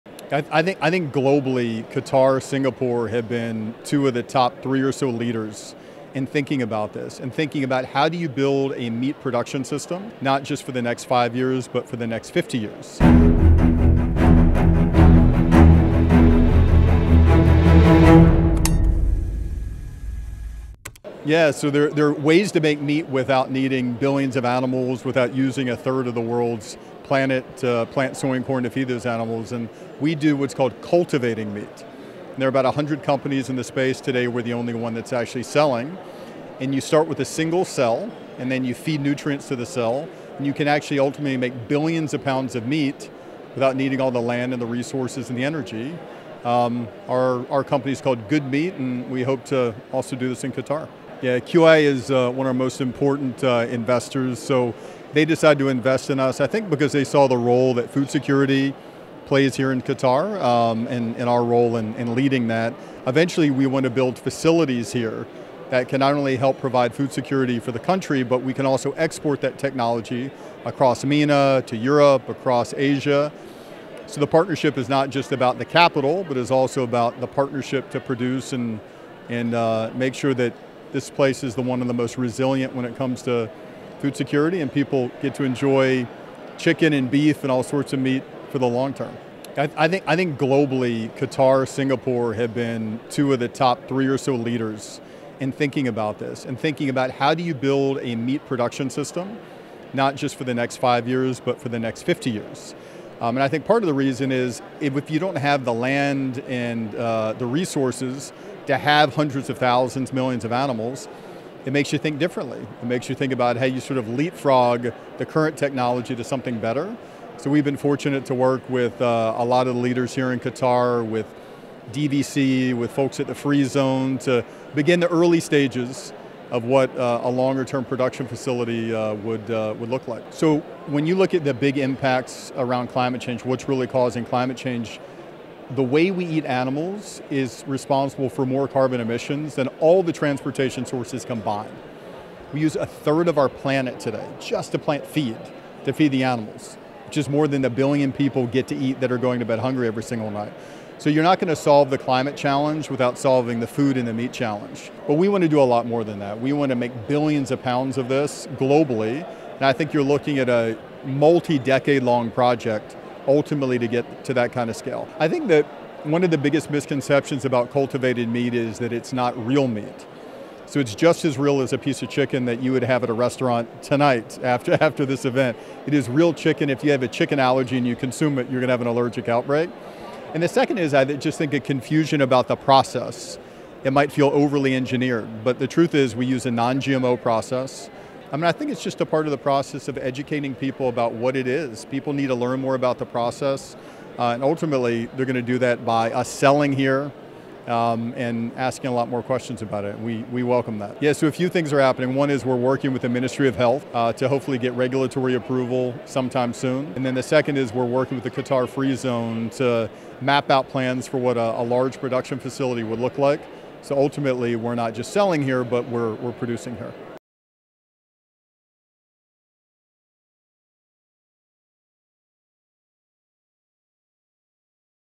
GOOD Meat: Cultivating meat without animals
How do you cultivate meat without using billions of animals?

Josh Tetrick, CEO of GOOD Meat, tells Doha News about the ways in which meat can be produced by feeding nutrients to cells, as well as the success of his company and their latest partnership deal with QIA.